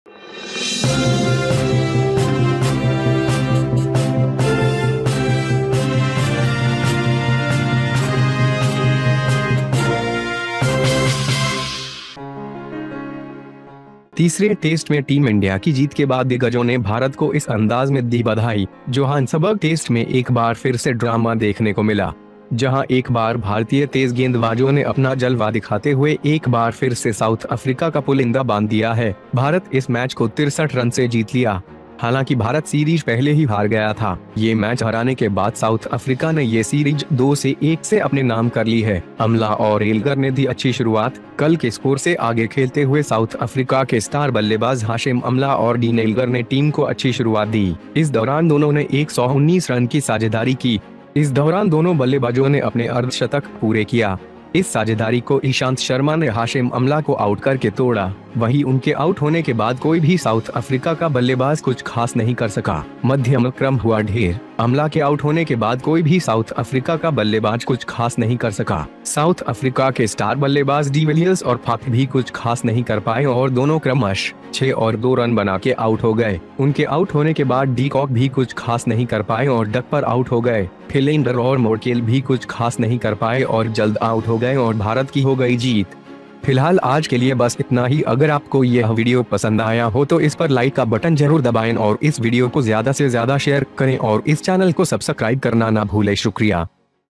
तीसरे टेस्ट मैच में भारत की हुई शानदार जीत.
After team India's victory in the third Test, veterans congratulated India on this pattern, in the Johannesburg Test, once again the drama was seen. Where once the Indian fast bowlers have once again strained the South Africa trophy after showing their flaw, India won the match by 63 runs. Although India had already lost the series, after defeating the match, South Africa has made it to the series 2-1, Amla and Elgar have given good start, ahead of yesterday's score, South Africa's star batsman Hashim Amla and Dean Elgar gave the team a good start, during which they shared a 119-run partnership. During this both the batsmen completed their half-century. This partnership was broken by Ishant Sharma by dismissing Hashim Amla, and after the dismissal, none of South Africa's batsmen could do anything special.

AK News